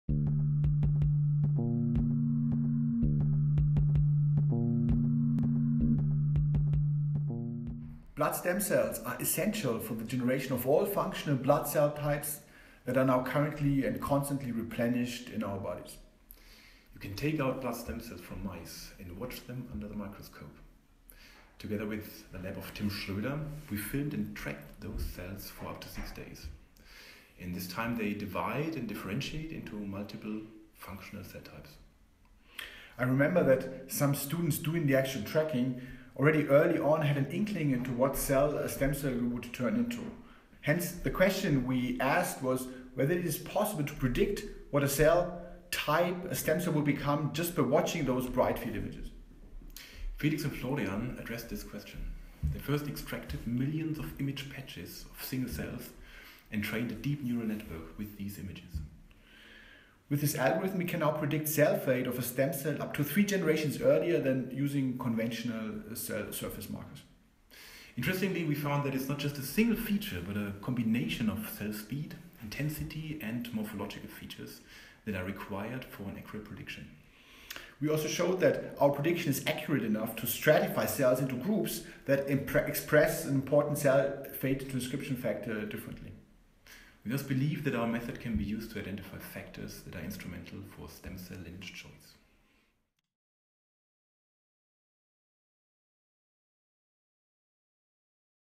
Deep Learning Predicts Stem Cell Development
Autonomous driving, automatic speech recognition, and the game Go: Deep Learning is generating more and more public awareness. Scientists at the Helmholtz Zentrum München and their partners at ETH Zurich and the Technical University of Munich (TUM) have now used it to determine the development of hematopoietic stem cells in advance. In ‘Nature Methods’ they describe how their software predicts the future cell type based on microscopy images. Here Fabian Theis and Carsten Marr explain how it works!